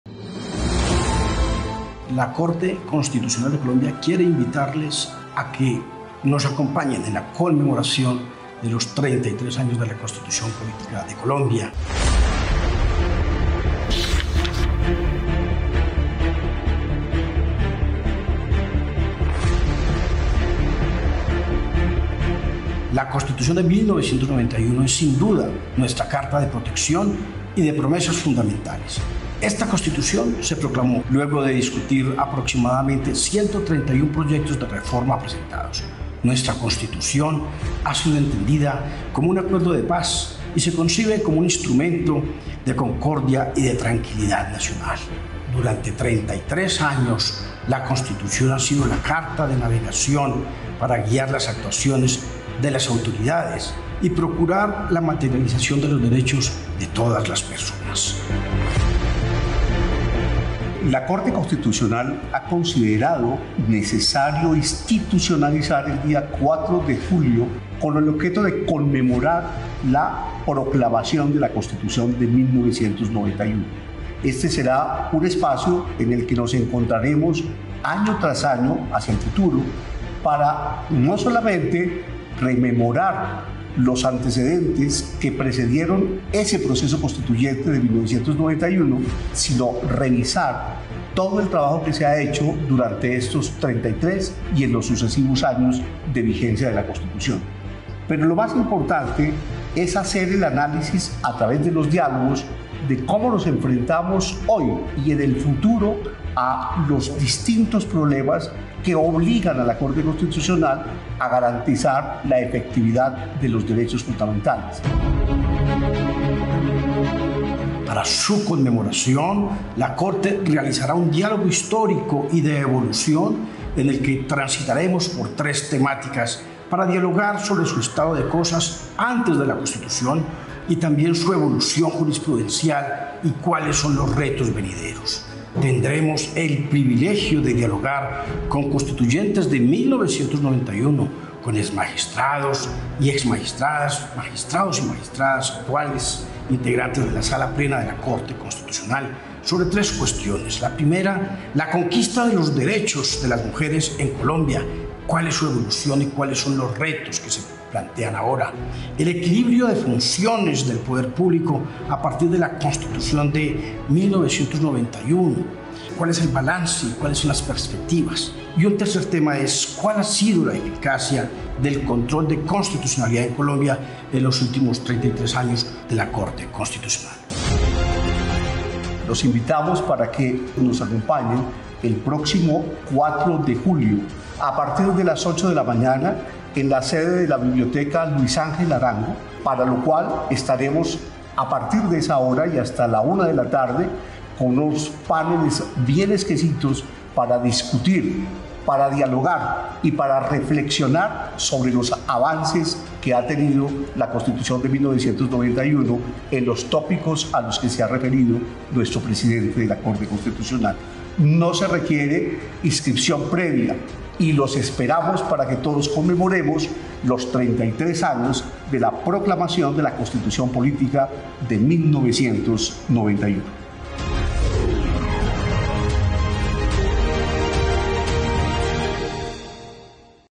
Invitación: Conmemoración de los 33 años de la Constitución Política de 1991
Un dialogo histórico y de evolución

Este dialogo se realizará de forma presencial el 🗓️ 4 de julio de 2024, desde las 🕦8:00 am hasta la 1:00 pm, en el Salón B, Centro de Eventos, Biblioteca Luis Ángel Arango (Bogotá).
🎟️ Entrada libre (sin previa inscripción)